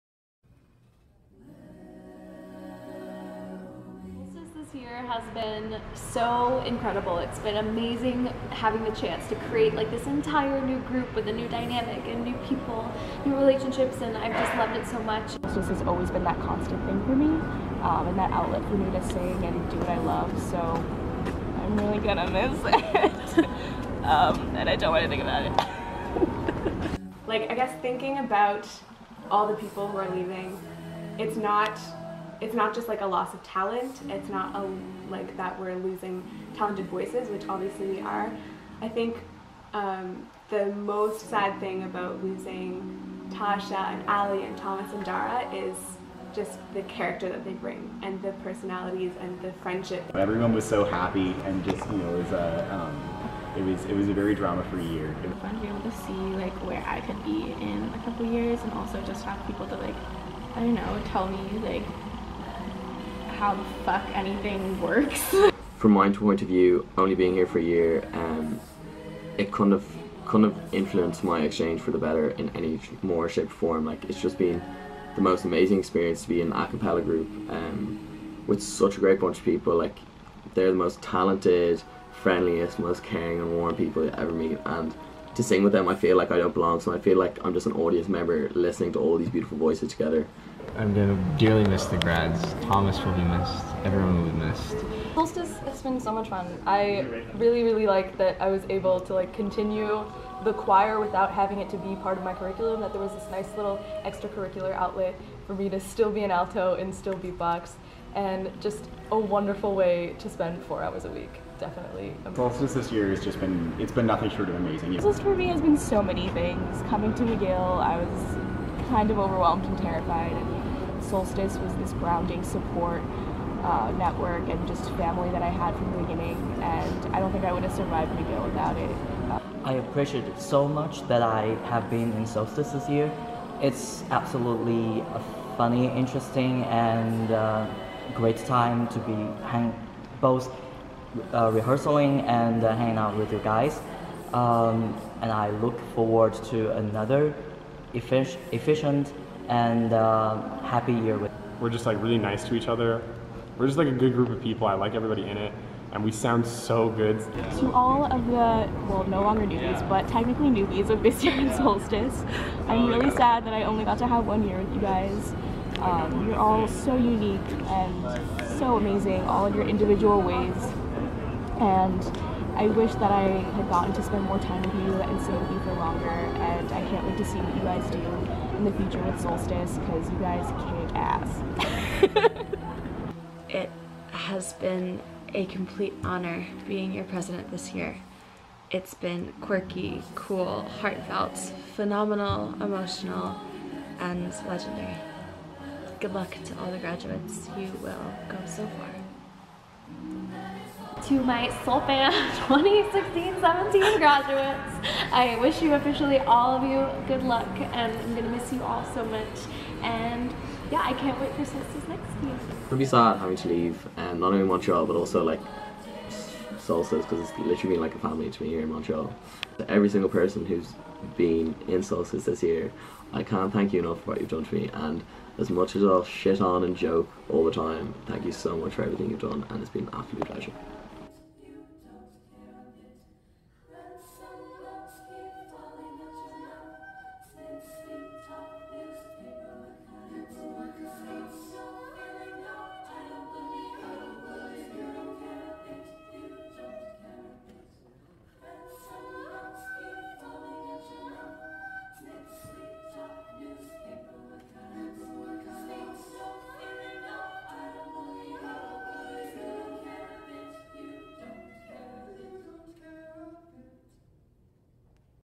2016//2017
the soul-fam touching on another incredible year of a cappella singing. a special shoutout to our lovely graduates; allie, darragh, tasha and thomas.
it's been a legendary time to say the very least! wishing everybody a lovely end of term.